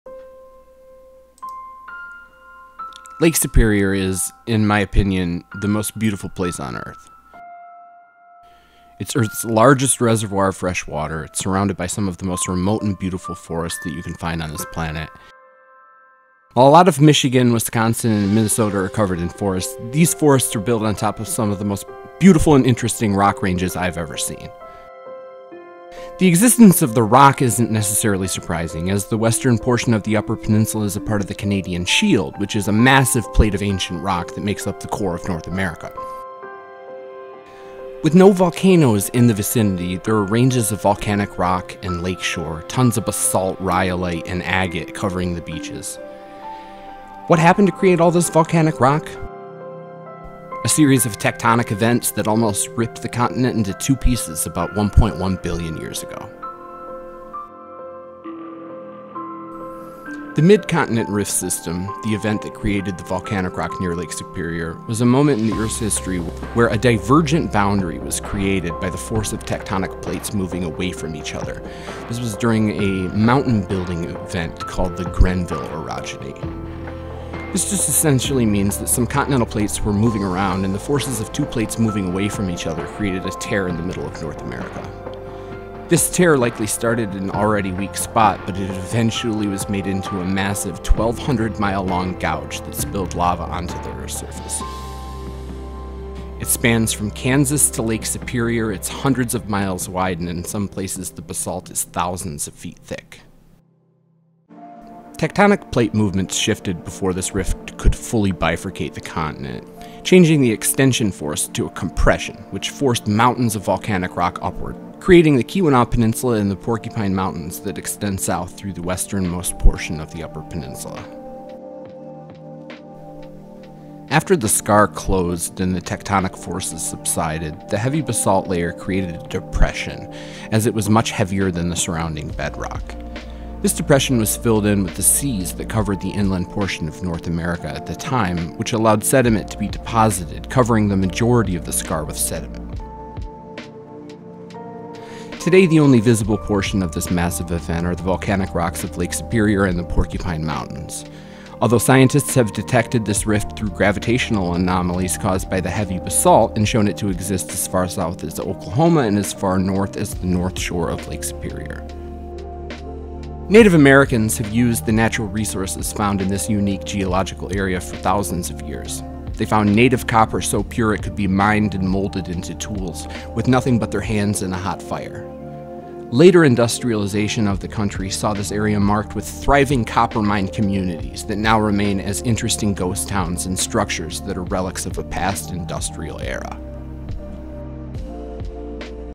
Is Lake Superior Volcanic?
1.1 billion years ago, the Midcontinent Rift system almost tore North America into two pieces. The scar of this event is visible today around Lake Superior, notably in the Porcupine Mountains and the Keweenaw Peninsula or Michigan's Upper Peninsula.